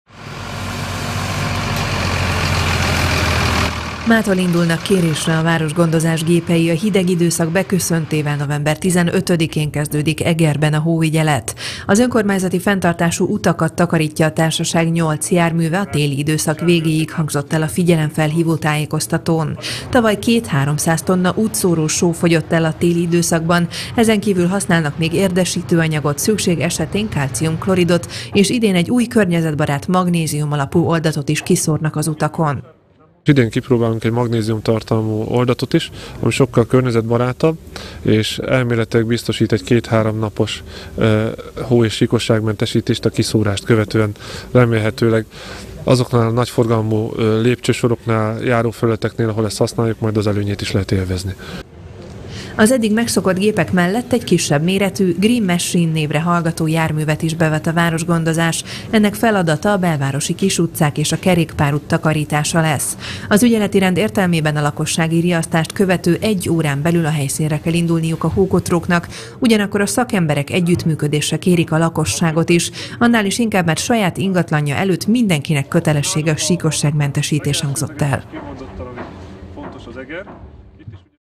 Hóügyelet - 2016.11.15.
Érkezik a tél, indul a hóügyelet. A Városgondozás Eger Kft. nyolc géppel gondoskodik a csúszásmentes utakról. Idén egy új, környezetbarát szóróanyagot is kipróbálnak majd.